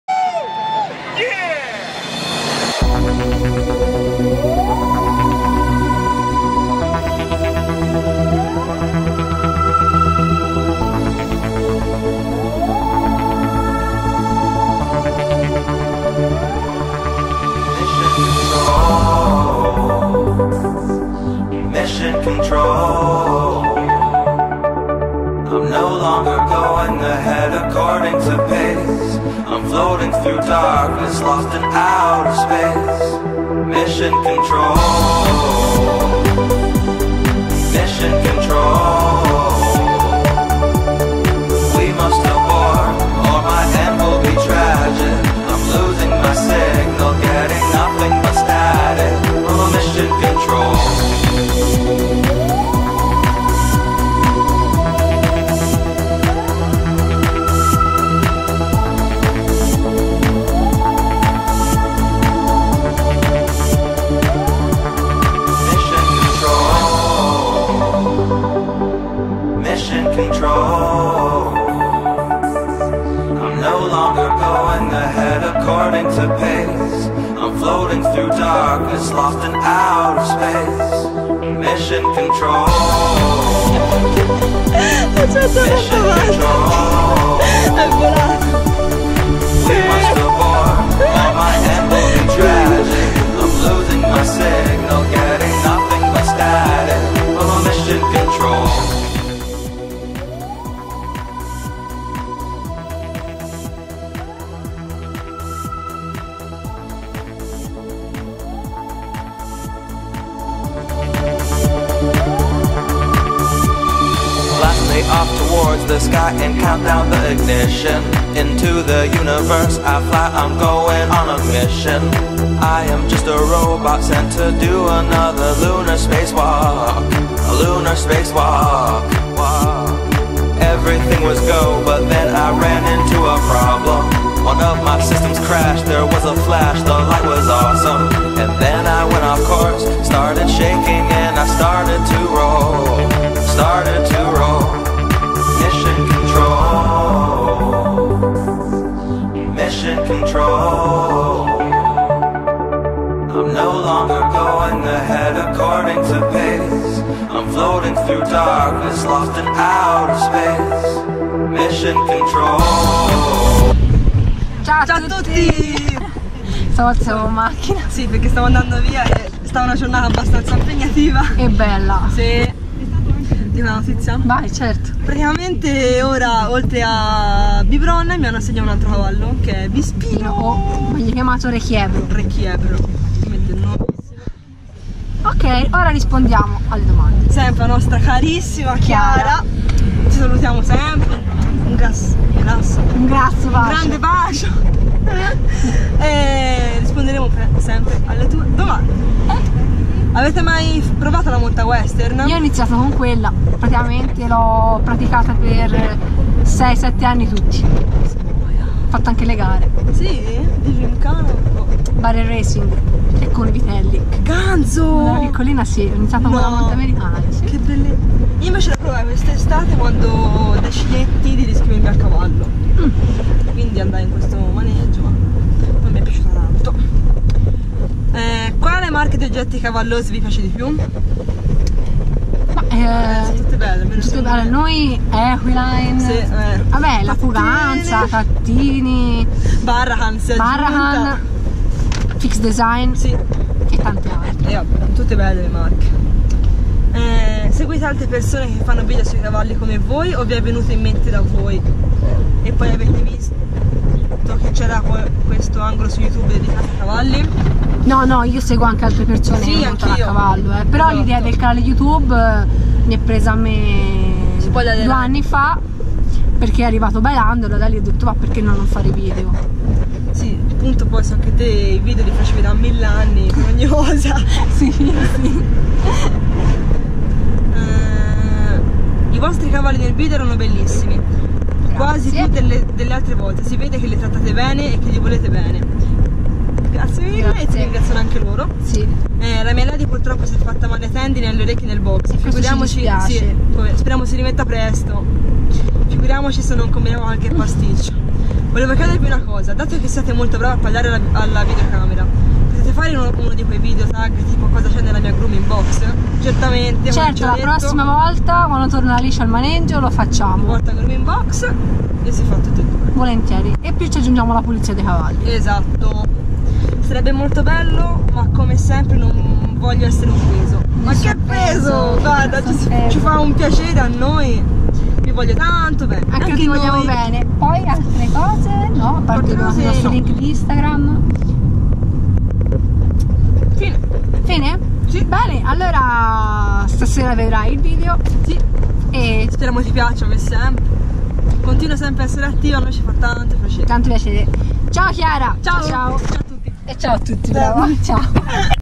Alta Scuola Spagnola part-10 Video Diary 7 // Rispondiamo a Chiara
Giornata Super bella! Alice oltre a Biberon ha montato Vispino e il mio Bailando! Come sempre cavalli spettacolari. Felice di passare giornate cosi insieme ad Alice. Aspettiamo tantissime domande da voi! Buona Visione , Video con Gopro Hero 5 Instagram Elena Instagram Alice